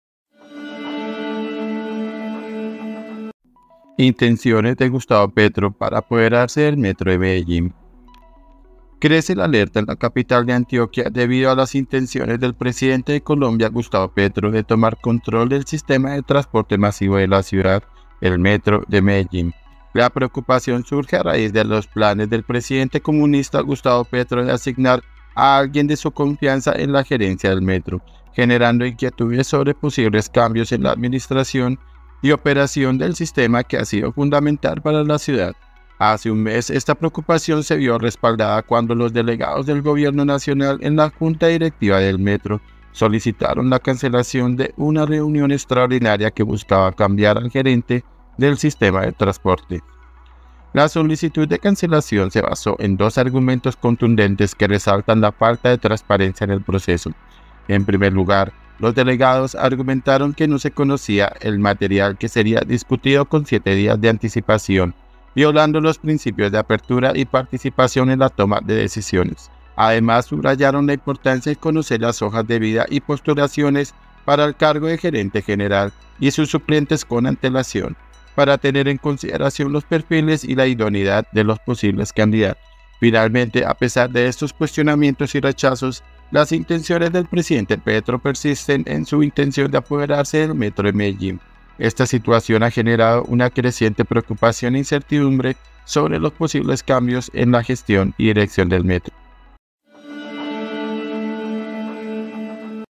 ADVERTENCIA 🚨 INTENSIONES DE GUSTAVO PETRO PARA APODERARSE DEL METRO DE MEDELLÍN
Crece la alerta en la capital de Antioquia debido a las intenciones del presidente de Colombia, Gustavo Petro, de tomar control del sistema de transporte masivo de la ciudad, el Metro de Medellín.
IFM NOTICIAS